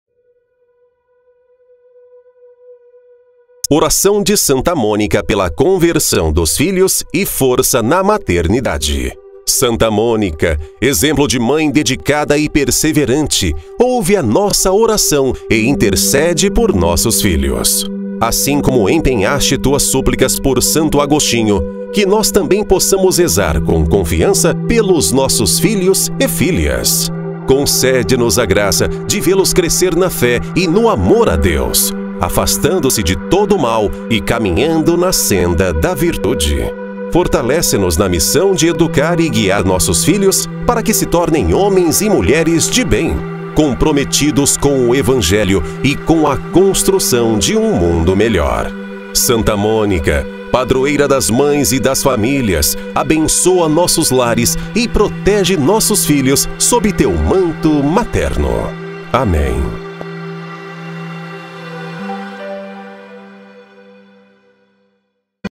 Oração de Santa Mônica pela conversão dos filhos e força na maternidade
Santa Mônica, exemplo de mãe dedicada e perseverante, ouve a nossa oração e intercede por nossos filhos. Assim como empenhaste tuas súplicas por Santo Agostinho, que nós também possamos rezar com confiança pelos nossos filhos e filhas. Concede-nos a graça de vê-los crescer na fé e no amor a Deus, afastando-se de todo mal e caminhando na senda da virtude. Fortalece-nos na missão de educar e guiar nossos filhos, para que se tornem homens e mulheres de bem, comprometidos com o Evangelho e com a construção de um mundo melhor. Santa Mônica, padroeira das mães e das famílias, abençoa nossos lares e protege nossos filhos sob teu manto materno. Amém.